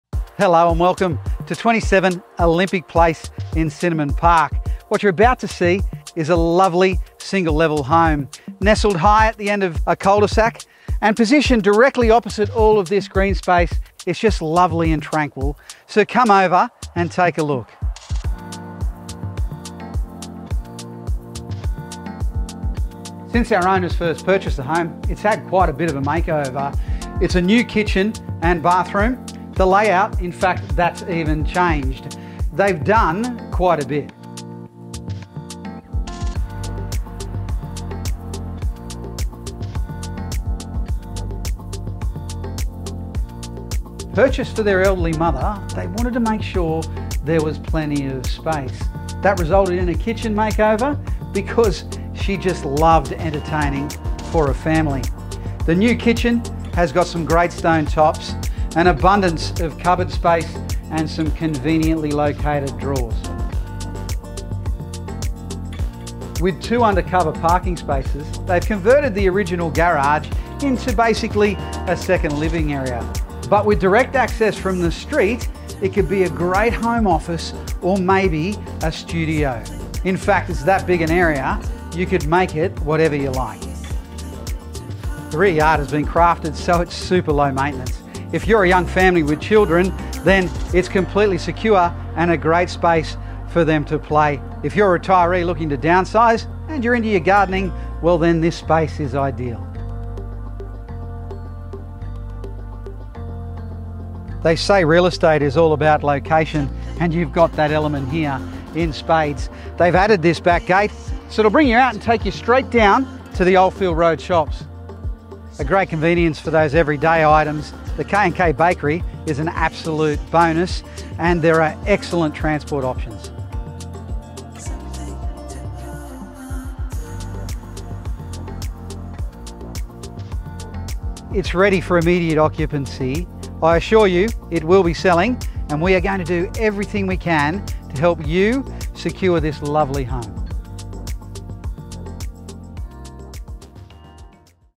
Perfect for young families and retirees
This lovely single-level home is situated in a private cul-de-sac on a spacious block of land. It is perfect for families who prefer a low-maintenance lifestyle while still being close to all the amenities the Centenary suburbs offer. The property is a short stroll from Sinnamon Park Village shops on Oldfield Road and less than 1.5km from the Mt Ommaney Shopping Centre.

The property is ready for immediate occupancy, and the main features of the property include (but are not limited to):

• Ideal separation between the master bedroom and the other bedrooms.
• A central kitchen with large stone benches and an abundance of storage.
• New kitchen with stainless-steel under-bench oven & a ceramic cooktop.